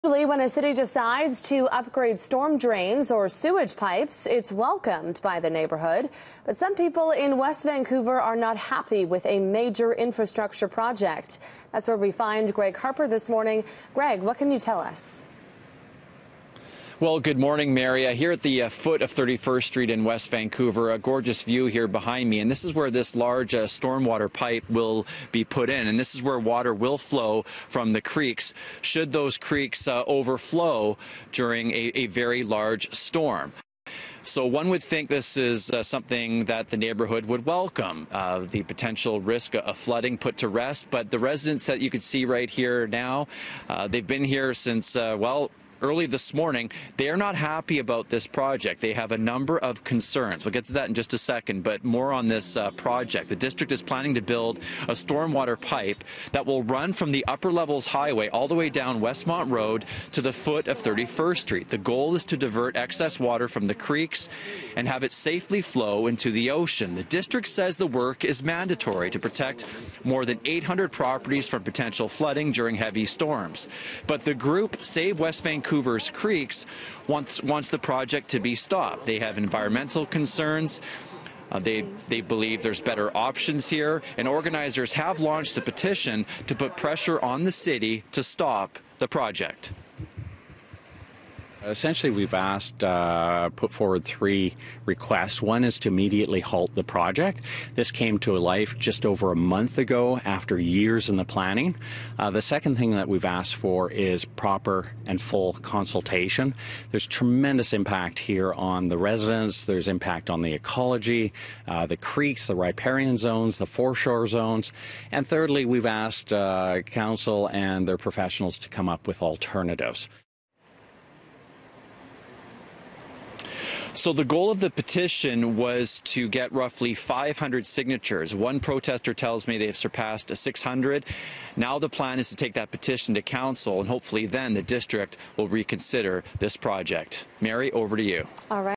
Protest over West Van stormwater project
Plan would link the British Properties to the ocean with new infrastructure.